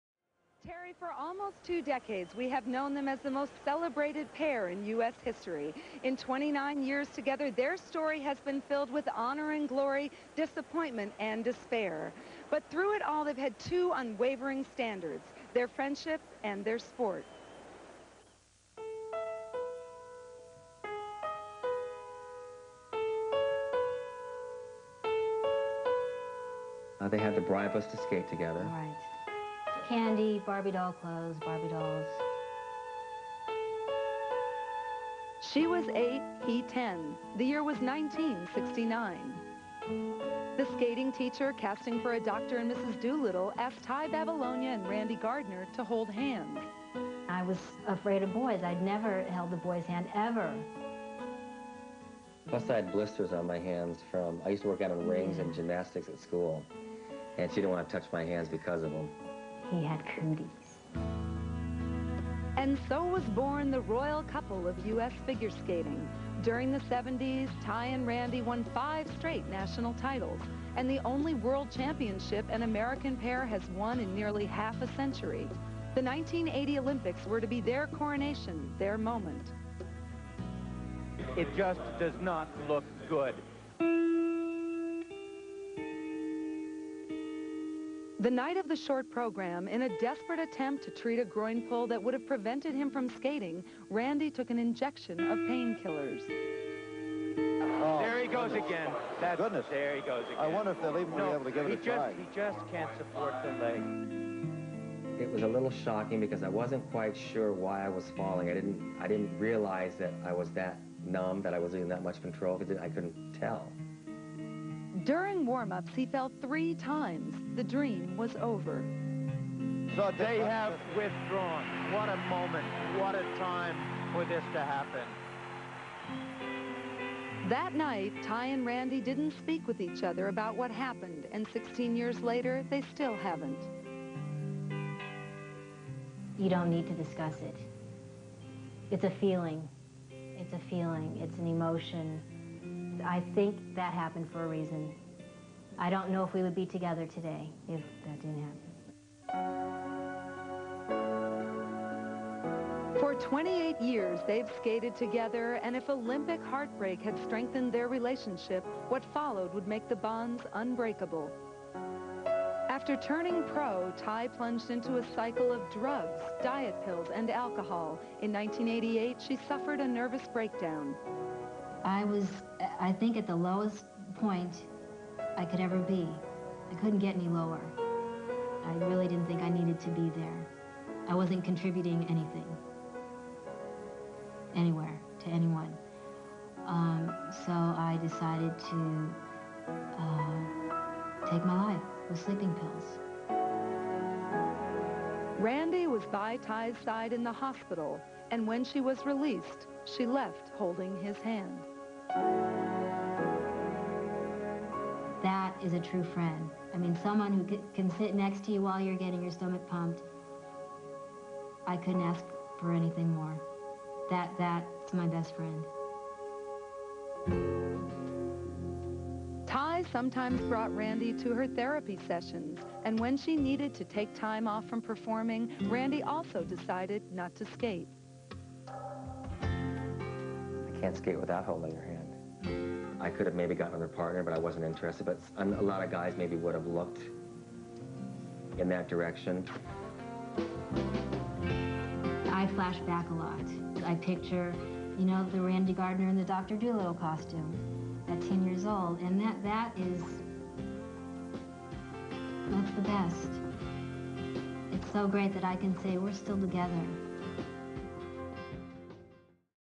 1996 US Professional Figure Skating Championships - Tai Babilonia/Randy Gardner (Profile)
1996 US Professional Figure Skating Championships
Profile on Tai Babilonia and Randy Gardner.

ABC Sports